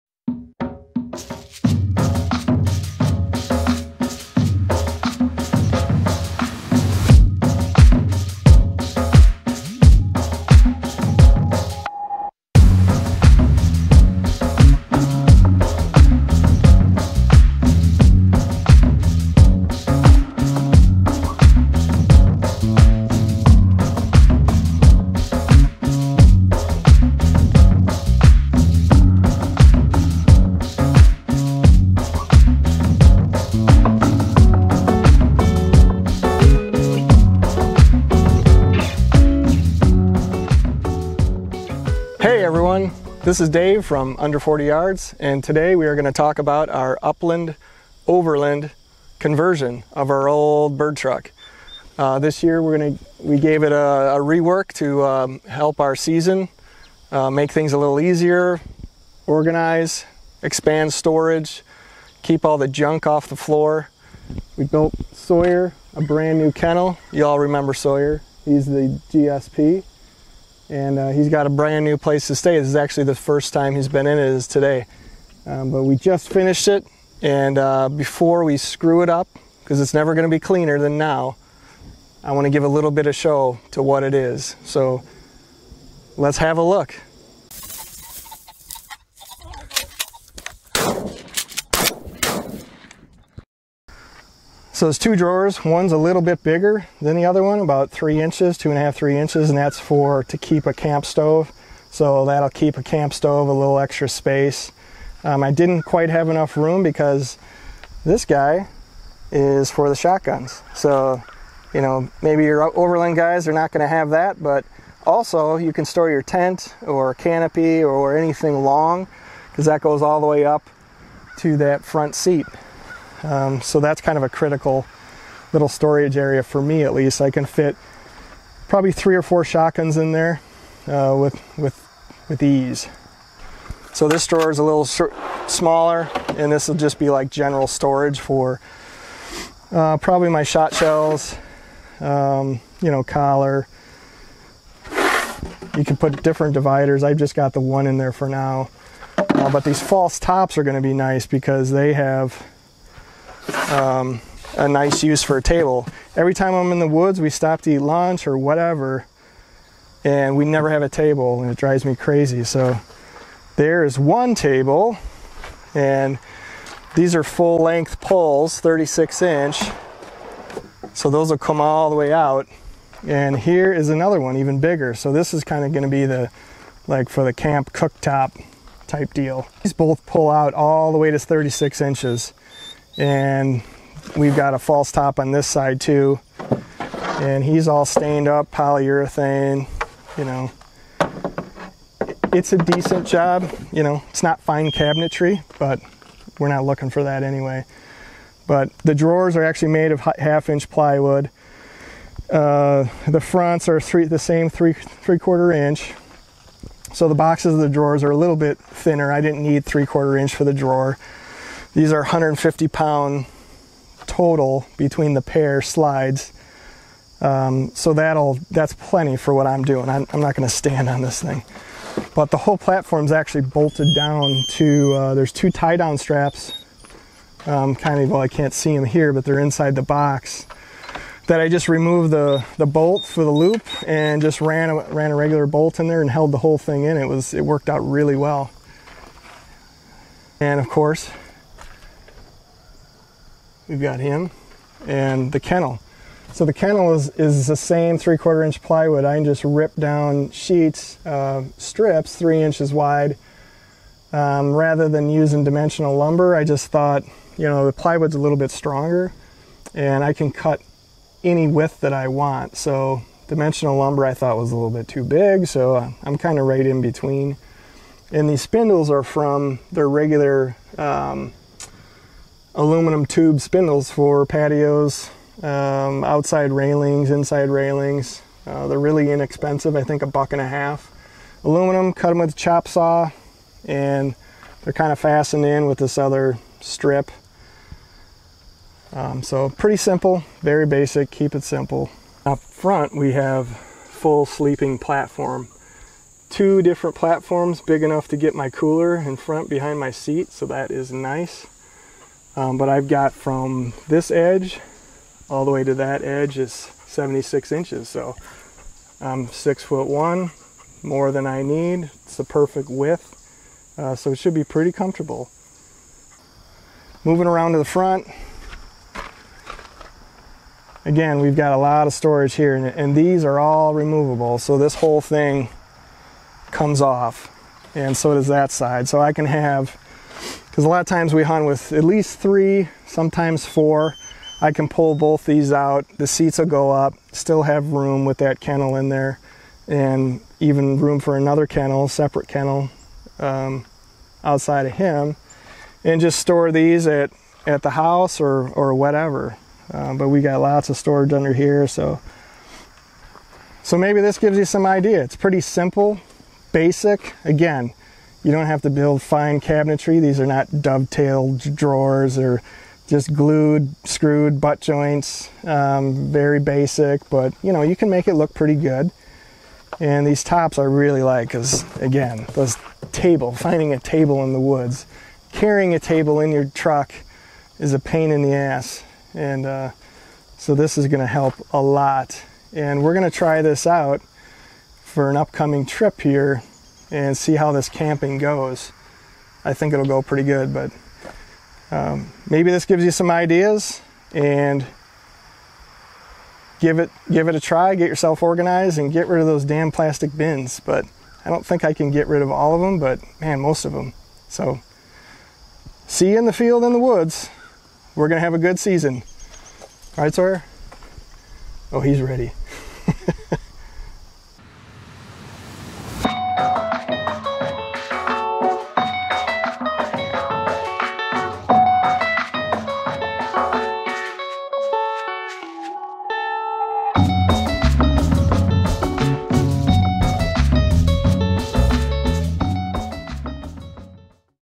Overland Hunting Drawers and Kennel
We've been banging around the woods for years in this old 1999 Ford Explorer. This season I decided to get more organized and built this drawer and custom kennel system. Simple design and build with basic tools. I don't own a table saw (wish I did) so we built this with a circular saw, miter box, sander, pocket screws and screw guns.

The drawers are 38" deep, with 36" drawer slides from Amazon. The platform and box is built from 3/4" ASX (aspen plywood) inexpensive and sands and stains easily.

If you like overland drawer systems with a hunting spin, you'll like this setup. Will serve to explore and camp in the uplands.